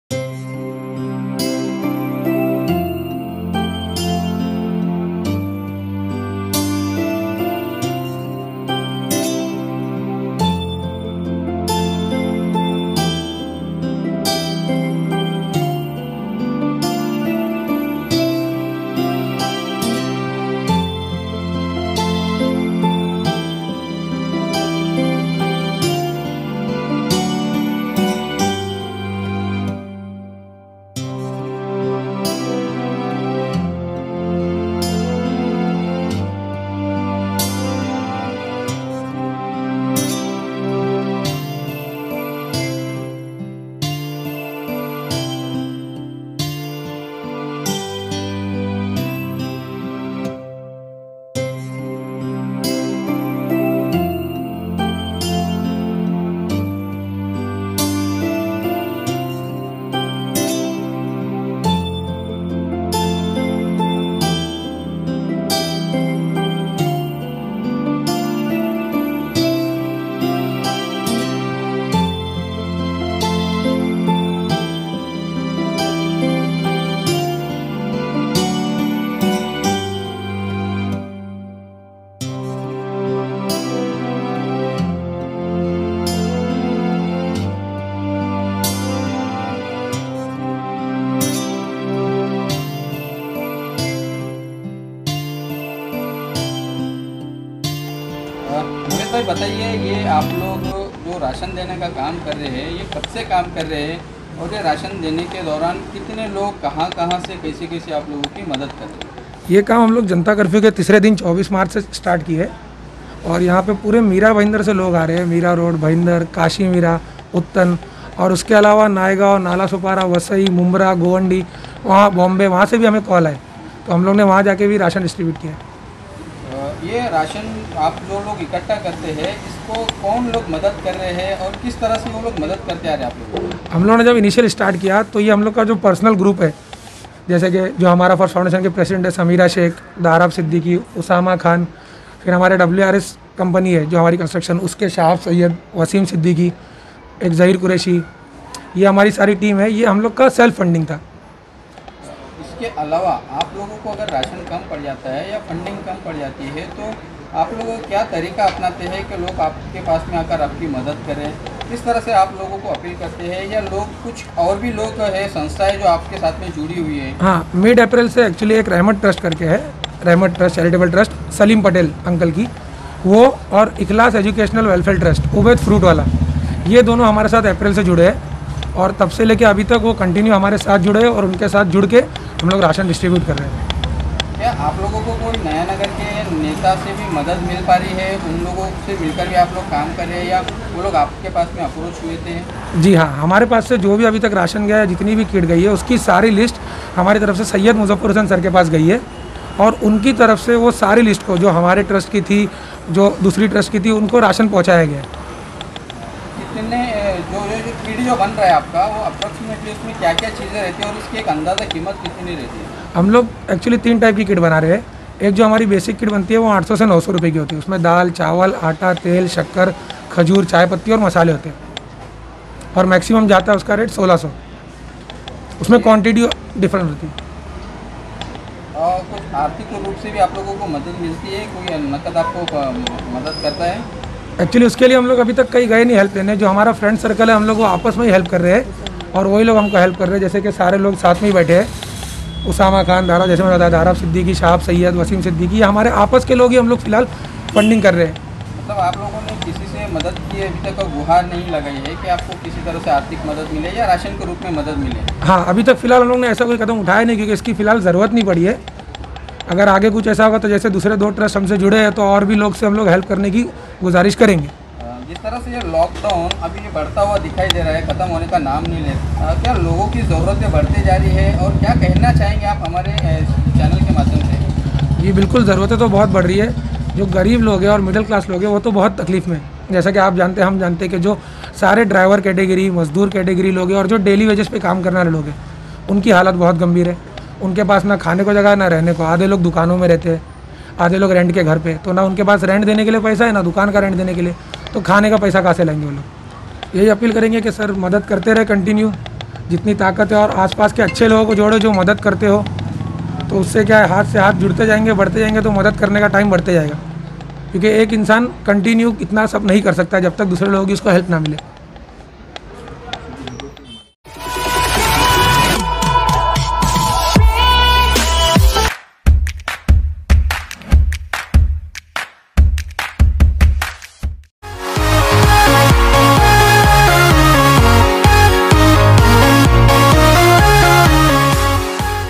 LockDown | Relief | Ration distribution | Nayanagar | Miraroad | Mira Bhaindar
हाथ से हाथ मिलाकर होरही है मदद
जरूरतमन्दों को घर घर जाकर पहुचाया जा रहा है राशन
नयानगर के युवाओं का राहत योगदान
अनेक संस्थाएं मिलकर बांट रही है राशन